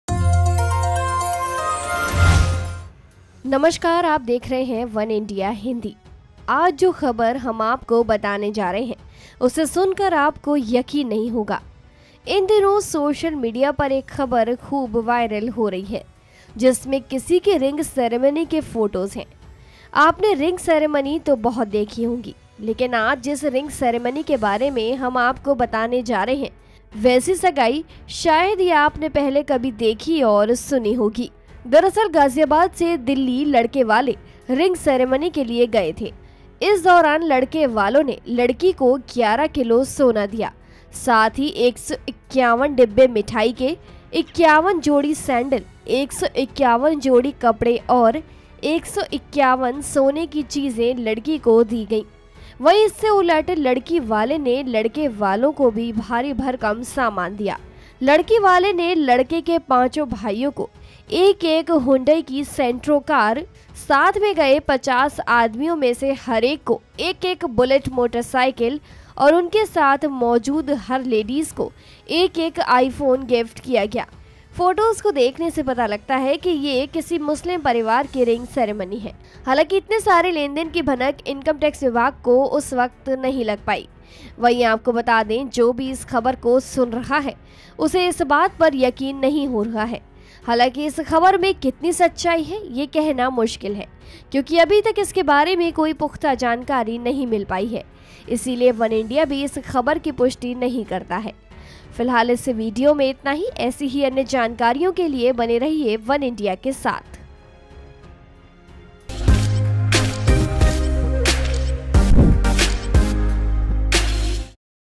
Delhi में धमाकेदार Ring Ceremony, Gift में दी 50 Bullet, 50 I phone, 5 Car & बहुत कुछ|वनइंडिया हिंदी
Delhi Ring Ceremony Video Goes Viral, 50 Bullet, 50 I phone 5 Car and many more gifted. These days a news is becoming very viral on social media. In which there are photos of one's ring ceremonies. Delhi boys from Ghaziabad went for ring ceremonies. During this period, the boy gave the girl 11 forts of gold, ... The companion was given 511 pairs shoes of 551 boxes of sweets, 151 pairs of clothes, and 151 pieces of gold were given to the girl.

इन दिनों सोशल मीडिया पर एक खबर खूब वायरल हो रही है । जिसमें किसी की रिंग सेरेमनी के फोटोज है । गाजियाबाद से दिल्ली लड़के वाले रिंग सेरेमनी के लिए गये थे। इस दौरान लड़के वालों लड़की को 11 किलों सोना दिया ,...  साथी ही 551डिब्बे मिठाई के 51जोडी सैंडिल, 151जोडी कपड़े, और 151  सोने की चीजें लड़की को दी गई ।